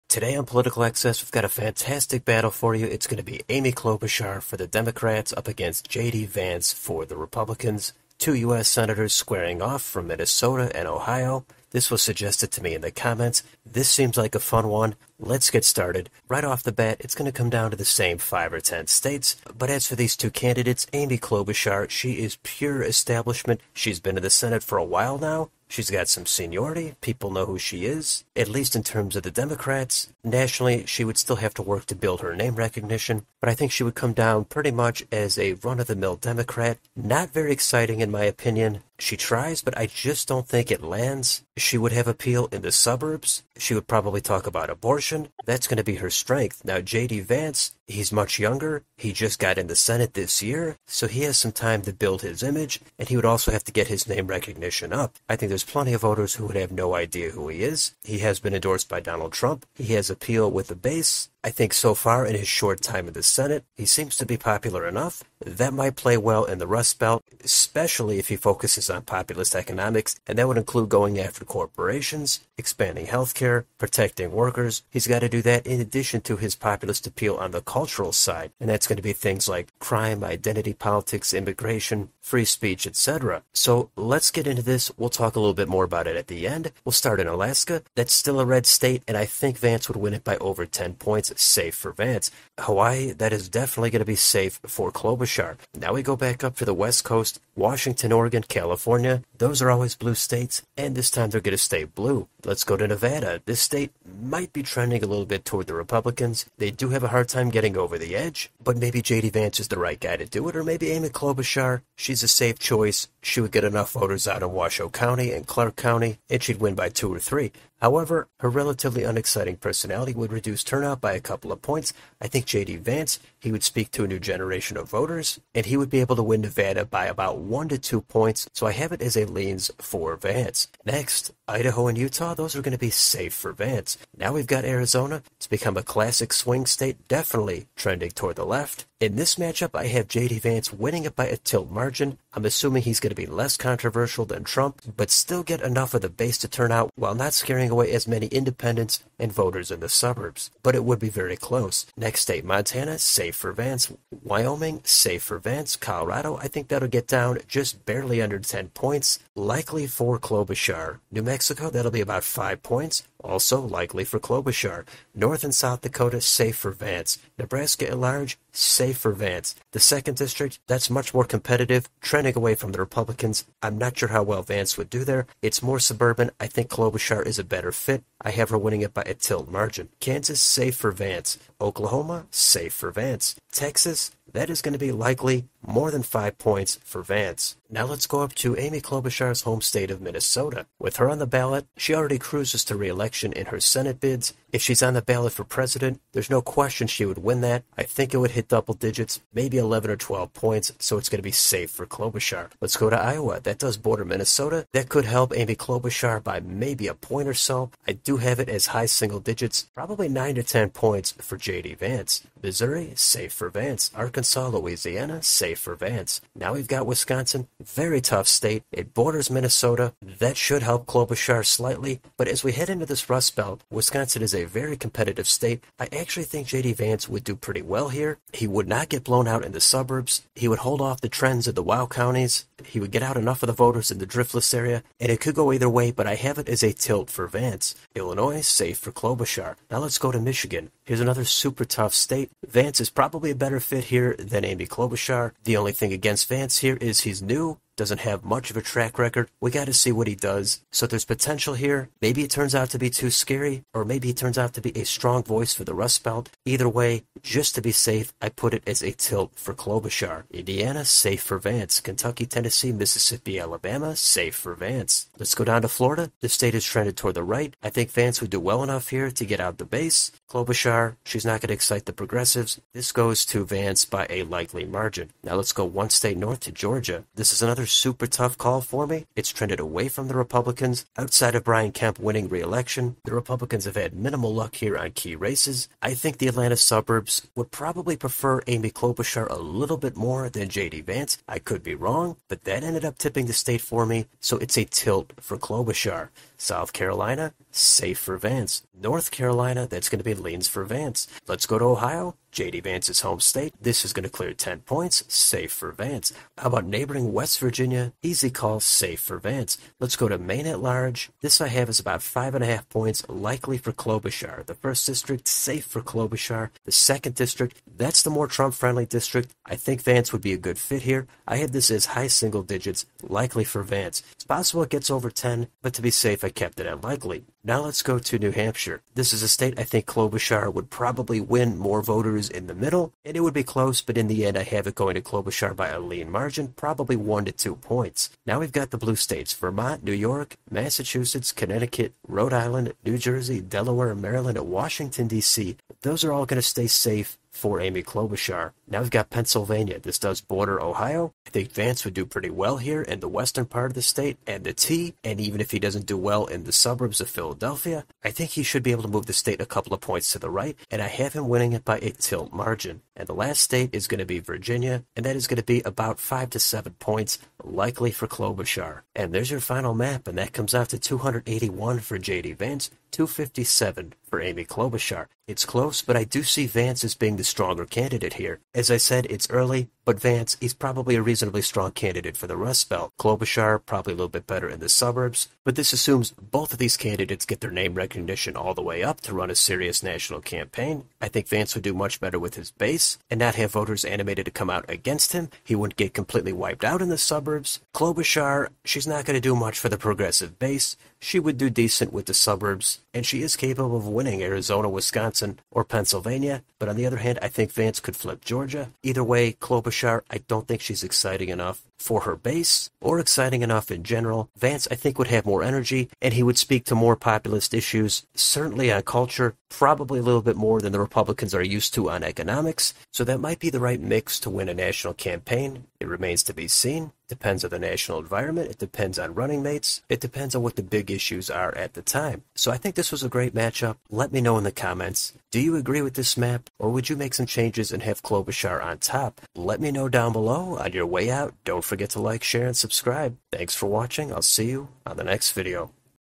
2024 Election | Amy Klobuchar vs. JD Vance
Hypothetical 2024 presidential election match-up between Amy Klobuchar for the Democrats and JD Vance for the Republicans.

Safe margins are over 10%, likely 5-10%, lean under 5%, tilt 1% or less.

00:00 - Intro / Background
01:26 - State Ratings
06:08 - Conclusion